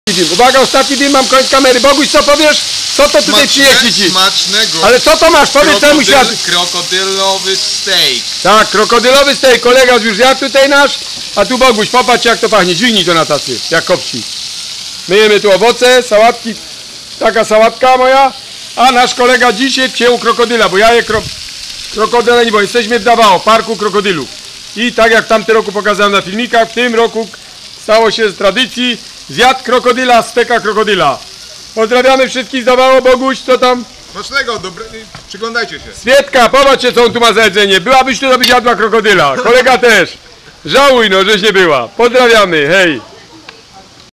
KROKODYL na talerzu
LUDZIE   po glodowce   nasz   ziomal    zjadł krokodyla   , taki apetyt   , taka moc    , zobaczcie   jak poradzili sobieze stekami krokodyla    , jestesmy w DAWAO   ,   park  krokodyli  ,   jemy jajak  krokodyla  , lody z krokodyla  i    takie dania  sewuja    , dla naszach smiałkow  BOGDAN    wie ze zyje   , i   on raczej nie gloduje   ,  wpadł do nas   w innych celach   , ale    z nim jest  wesoło   ,  tak zyjemy   w  MINDANAO    jestesmy sami   bialasy zostały na PALAWANIE  NA  zimnym browarku    ,  hej sai